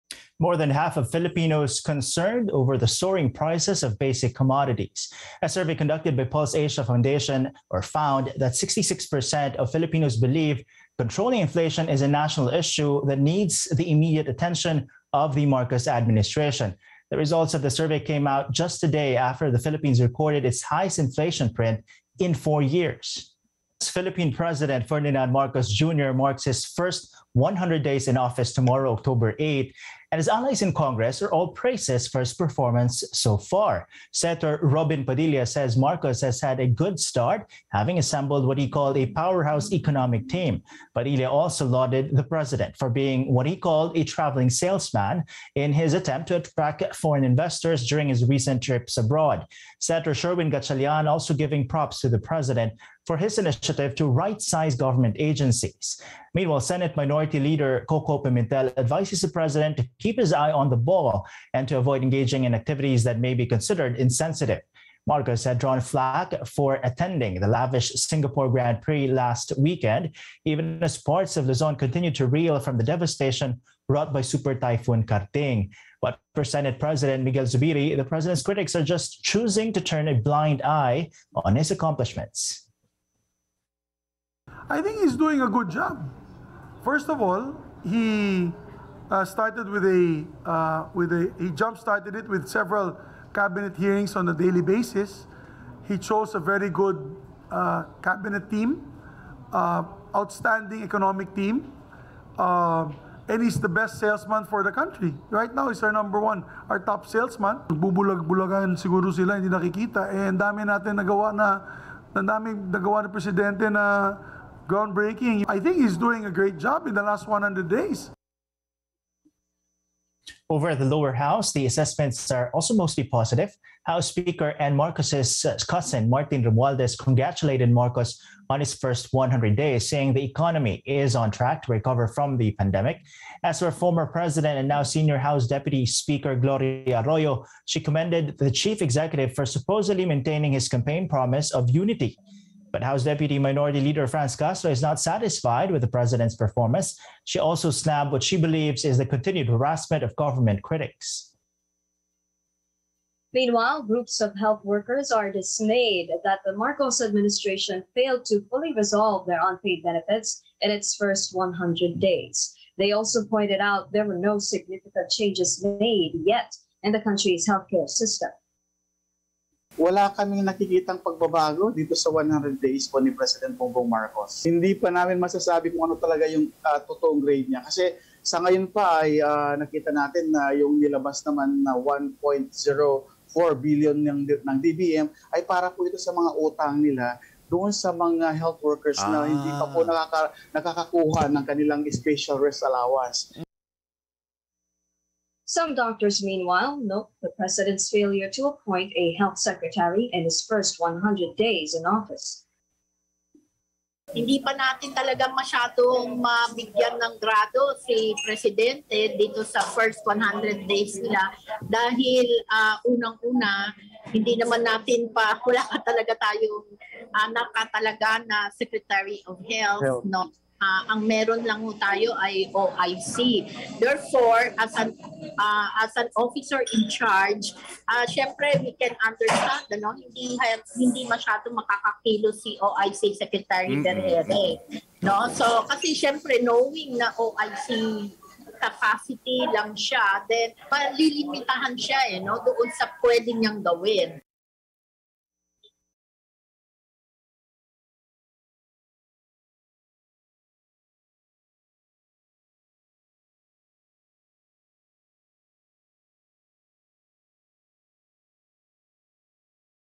Inflation still top concern for Filipinos -- Pulse Asia survey | ANC
Rundown: More than half of Filipinos are concerned over the soaring prices of basic commodities.